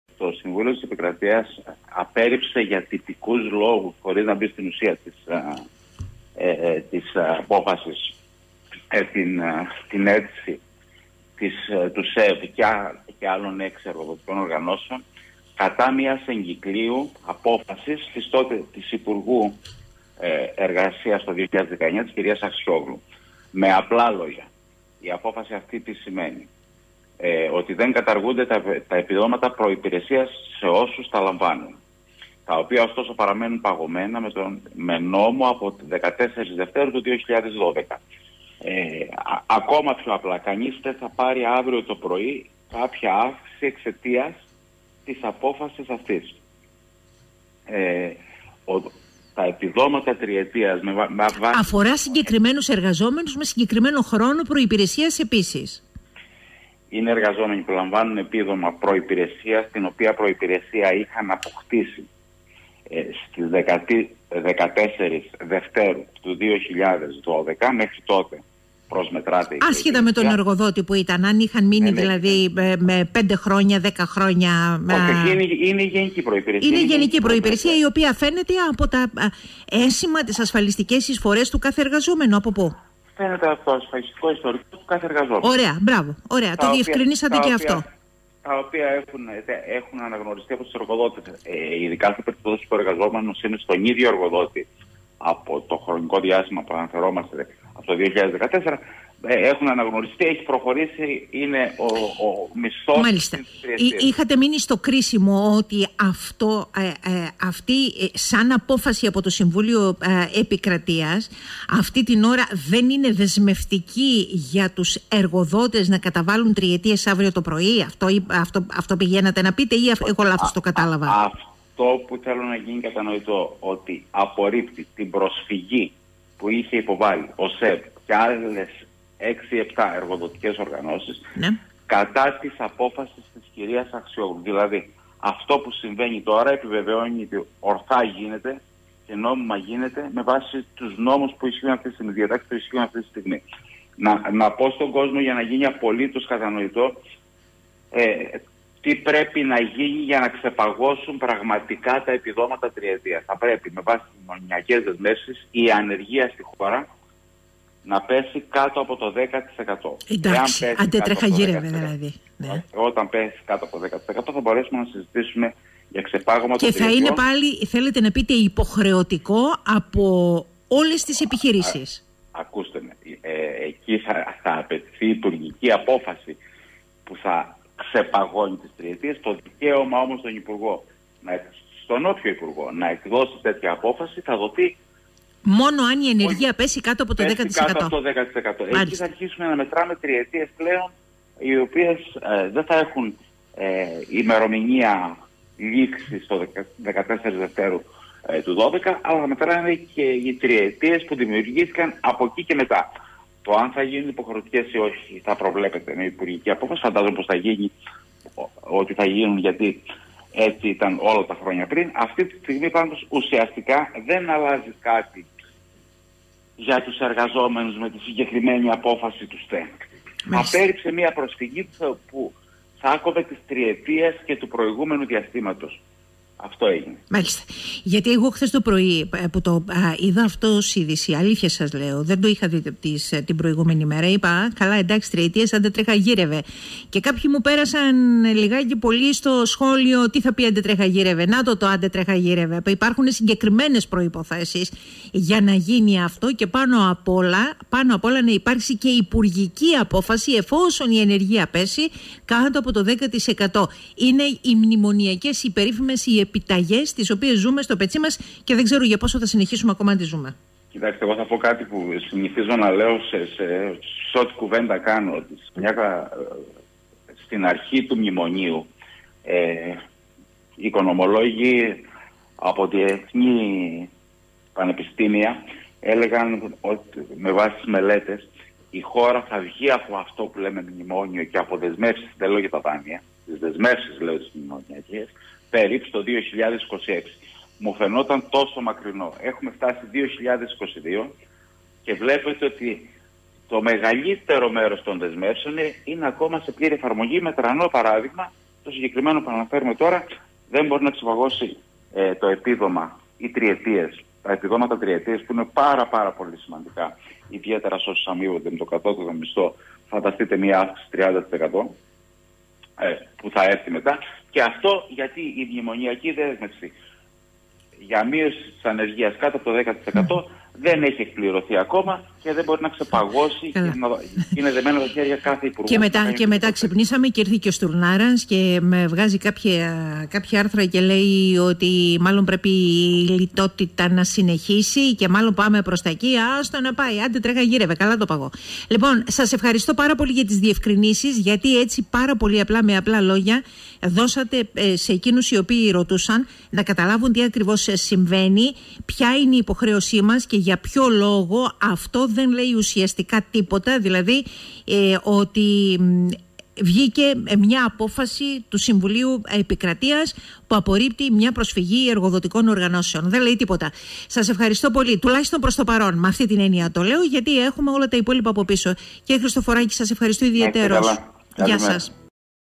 Μόνο με απόφαση Υπουργού και υπό προϋποθέσεις η καταβολή των τριετιών
Μην περιμένει κάποιος ότι αύριο το πρωί θα δει την αποτύπωση της απόφασης του ΣτΕ στο μισθό τους

Τα παραπάνω διευκρινίζει μέσω του 98.4 ο πρόεδρος των Φοροτεχνικών ηρακλείου Δημήτρης Χριστοφοράκης μεταξύ άλλων.
Το ΣτΕ απέρριψε προσφυγή εργοδοτικών οργανώσεων κατά της εγκυκλίου του υπουργείου Εργασίας του 2019 επί κυβέρνησης ΣΥΡΙΖΑ, η οποία όριζε ότι οι τριετίες που είχαν «παγώσει» το 2012 πρέπει να συνεχίσουν να καταβάλλονται.
Η ΕΝΥΠΕΚΚ, από την πλευρά της, υποστηρίζει ότι «το ΣτΕ απέρριψε για τυπικούς λόγους μόνο τις προσφυγές των βιομηχάνων και δεν αποφάνθηκε για την υποχρέωση των εργοδοτών να χορηγούν κανονικά τις τριετίες στον ιδιωτικό τομέα.
Το επίδομα προϋπηρεσίας, γνωστό και ως «τριετία», ισούται με 10% επί του κατώτατου μισθού. Μπορούν να καταβληθούν έως τρεις τριετίες κατ’ ανώτατο όριο.